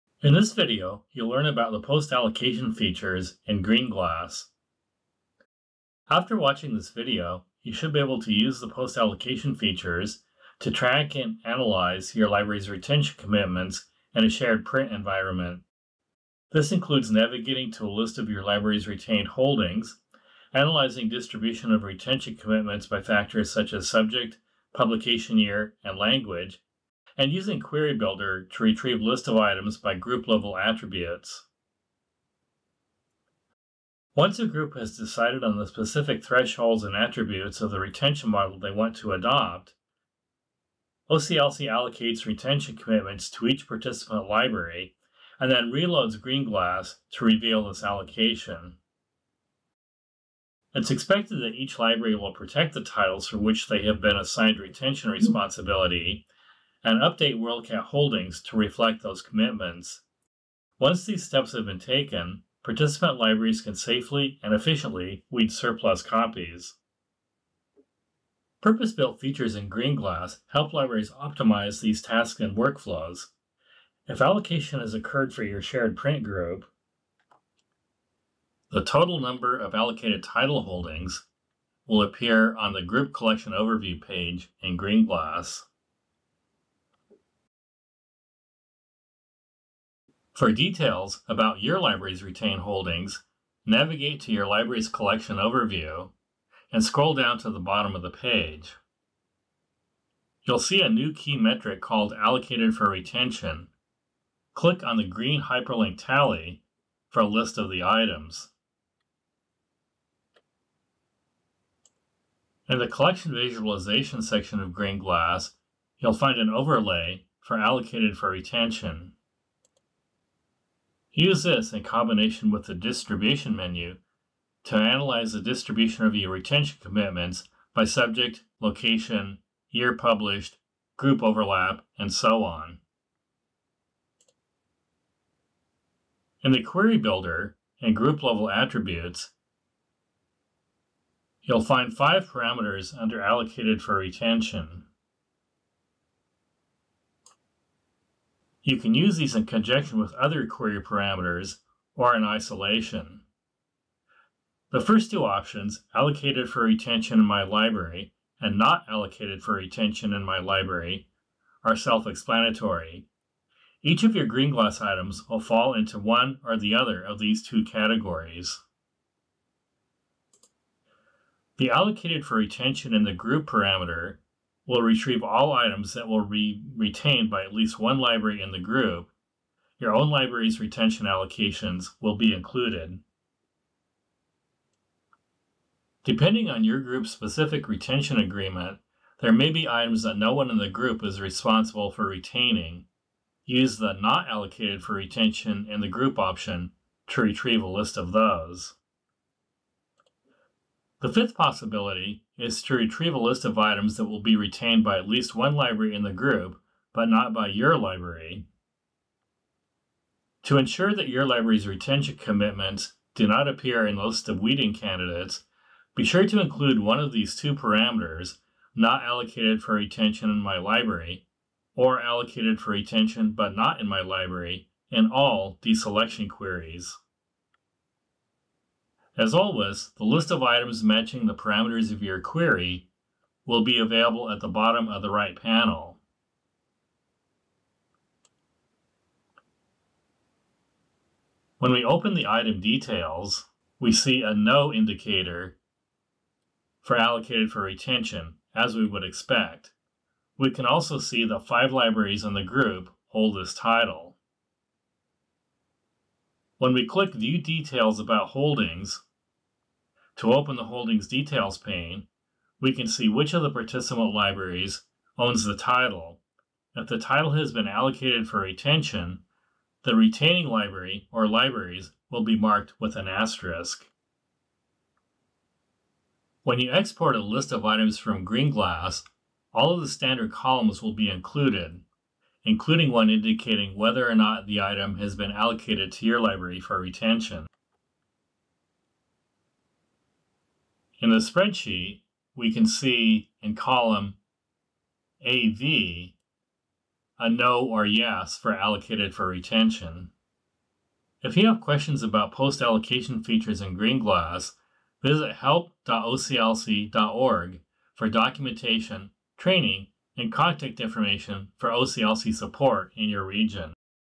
Post allocation features in GreenGlass
After watching this video, you should be able to use the post allocation features in GreenGlass to track and analyze your library’s retention commitments in a shared print environment.